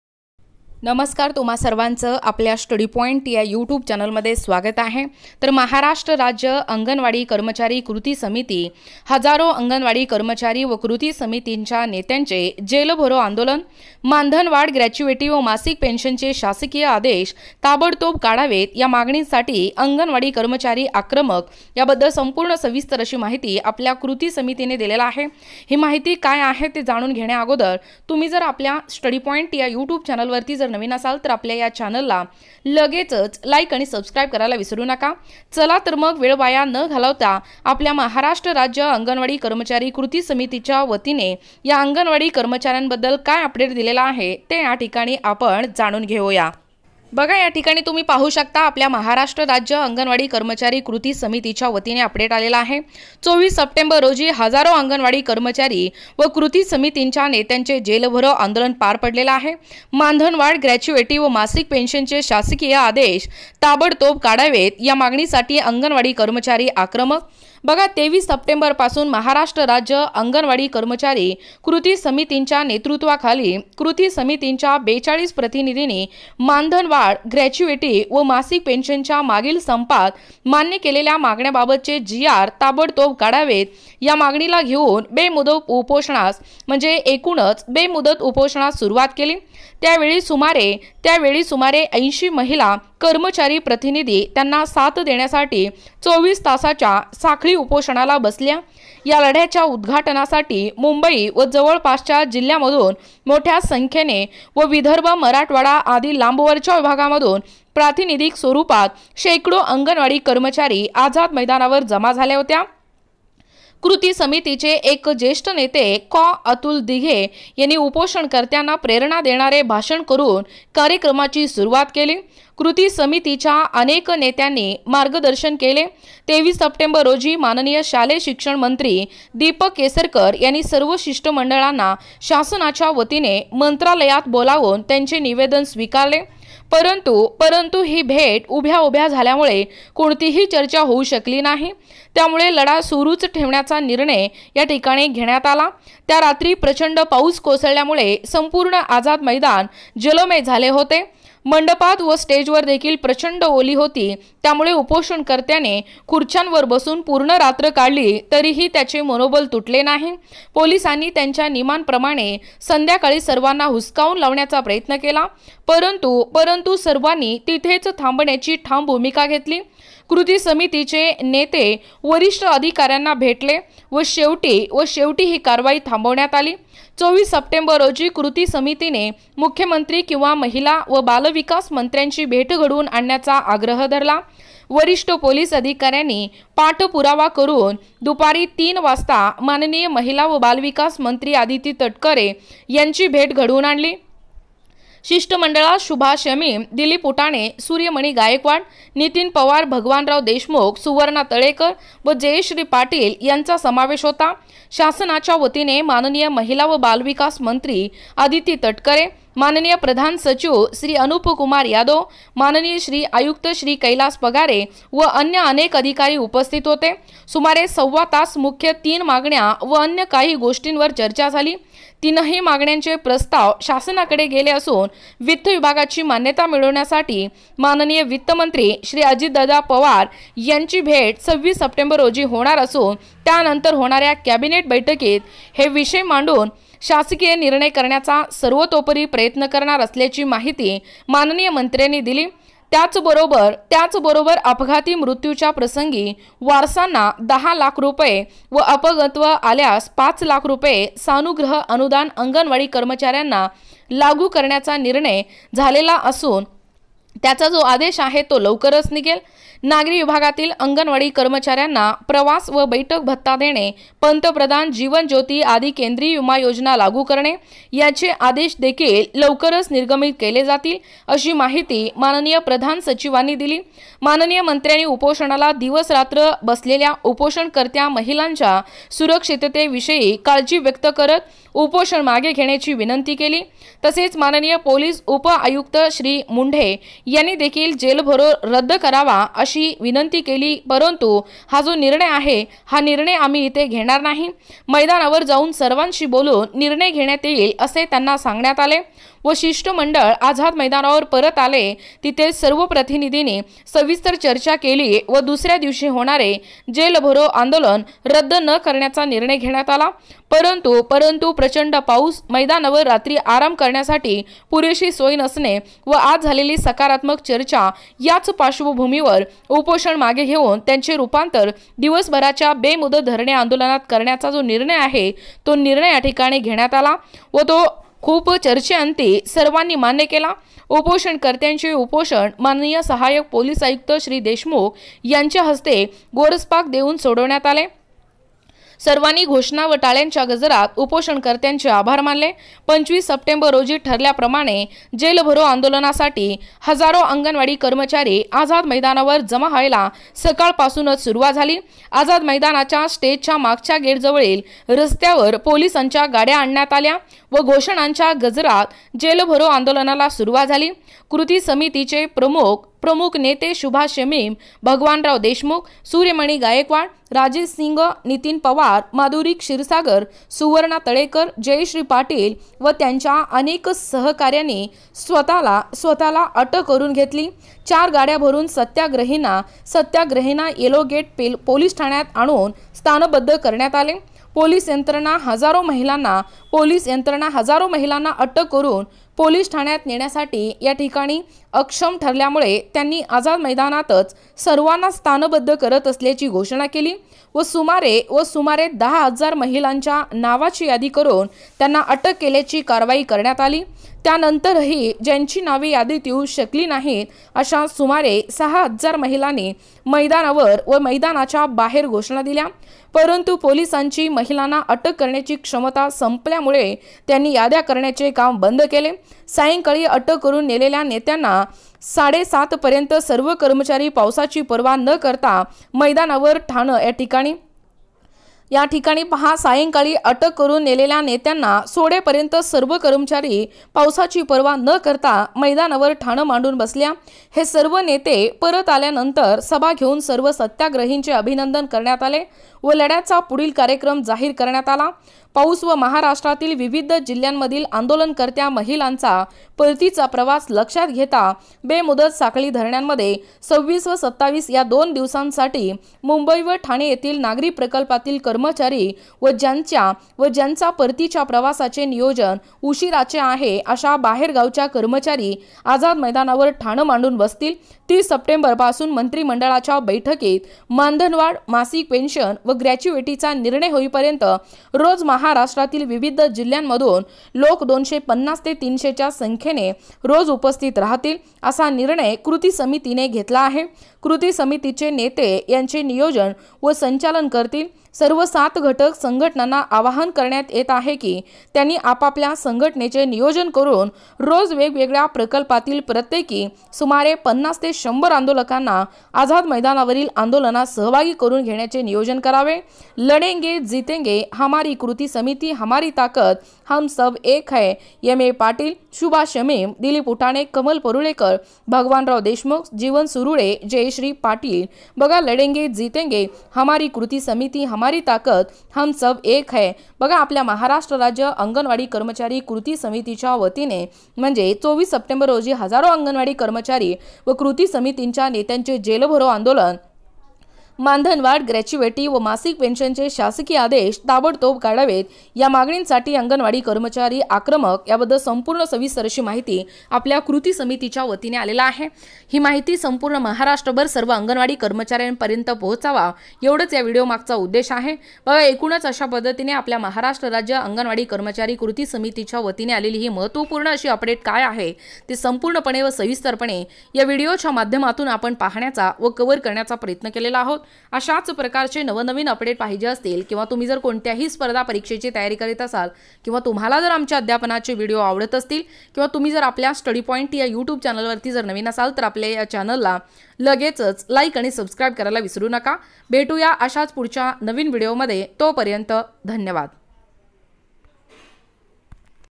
महाराष्ट्र राज्य अंगणवाडी कर्मचारी कृती समिती l Aganvadhi karmchari Andolan update today 2024....
महाराष्ट्र राज्य अंगणवाडी कर्मचारी कृती समिती

२४ सप्टेंबर रोजी हजारो अंगणवाडी कर्मचारी व कृती समितीच्या नेत्यांचे जेल भरो आंदोलन

मानधनवाढ, ग्रॅच्युईटी व मासिक पेन्शनचे शासकीय आदेश ताबडतोब काढावेत या मागणीसाठी अंगणवाडी कर्मचारी आक्रमक